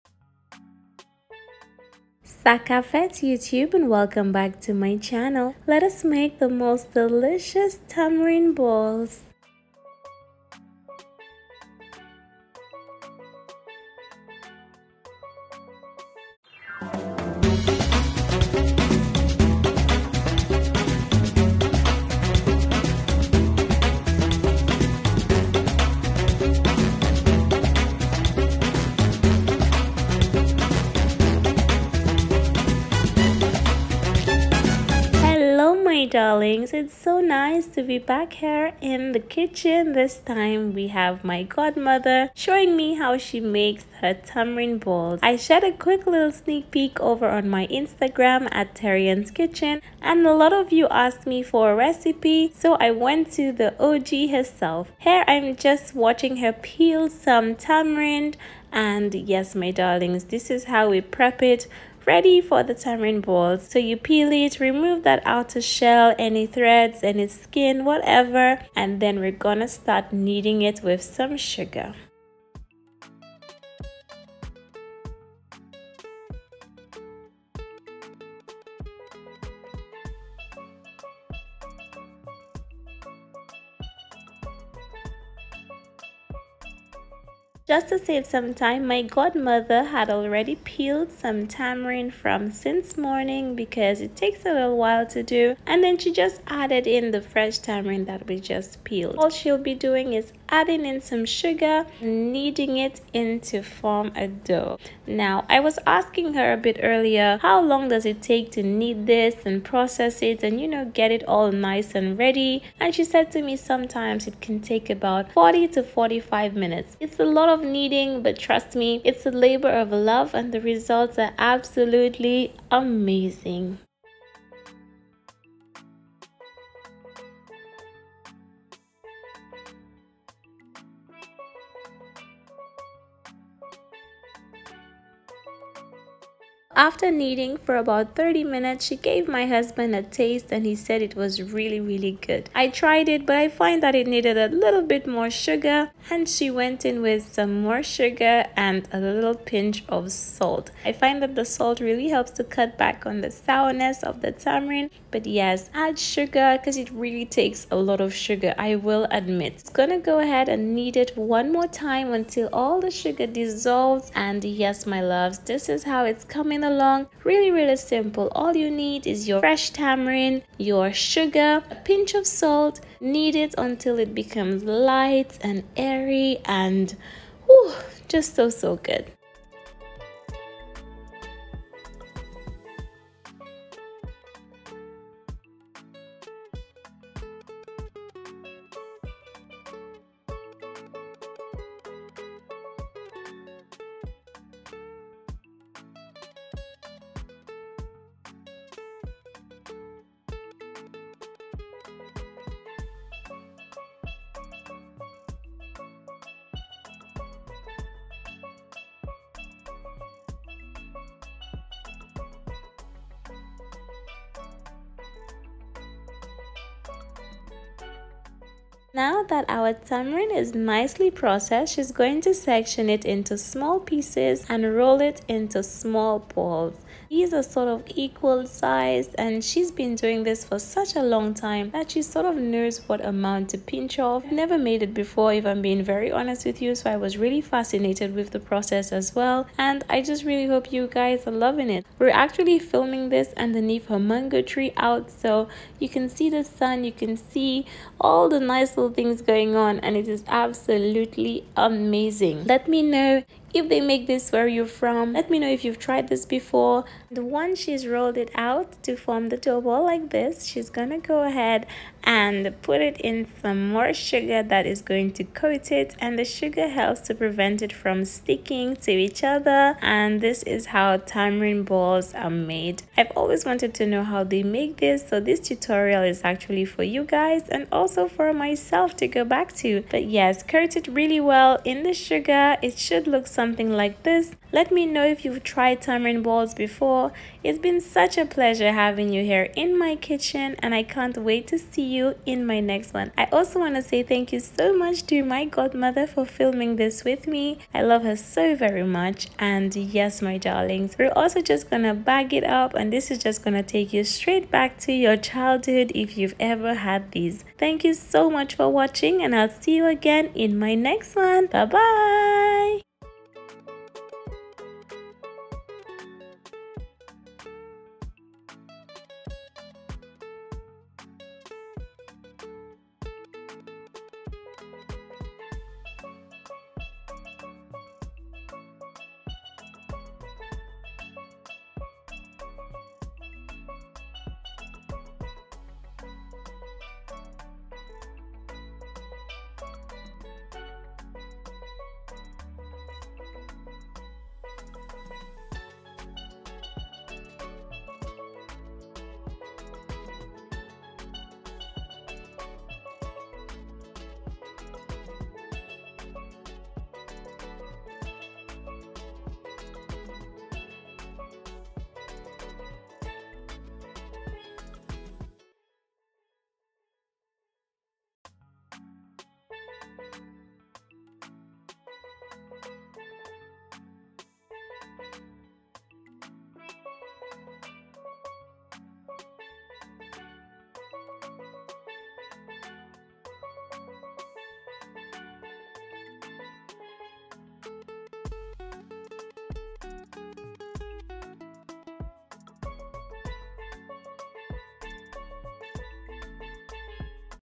Easy Tamarind Balls || A childhood treat that’s super delicious
Ingredients
Tamarind
Sugar
Pinch of salt
Vanilla Essence (optional- we didn’t use any)

Music Credits @chazz758